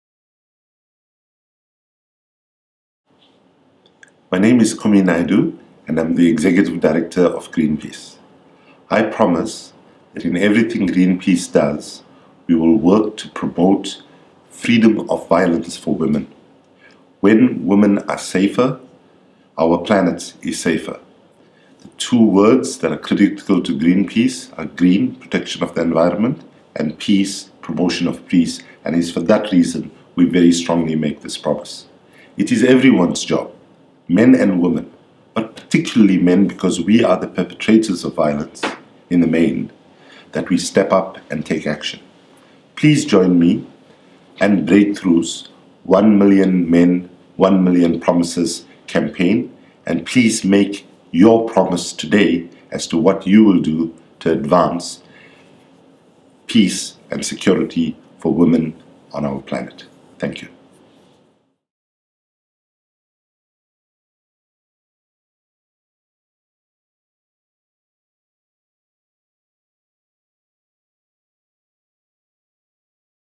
Kumi Naidoo Promises to Help End Violence Against Women
Kumi Naidoo, International Executive Director of Greenpeace, promises to help end violence against women.

One million promises calls on men worldwide to promise to take concrete action to end violence against women.

Men and women together have responded with outrage to recent dramatic acts of violence from Delhi to Cairo to Steubenville. We know that effective laws, courts, and cops are essential -- but they are not enough. We need individual and community action to challenge the habits and norms that perpetuate violence.

Together with our partners in South Africa, Brazil, Malaysia, and more, we will secure one million promises in one year, starting March 8, 2013 (International Women's Day). With men as leaders and partners, we can build a world in which women are safe — and in which all of us live freely, fully, and without fear.

Together we can build a safer world.

The video was later uploaded to the Breakthrough U.S. Youtube channel for archive purposes.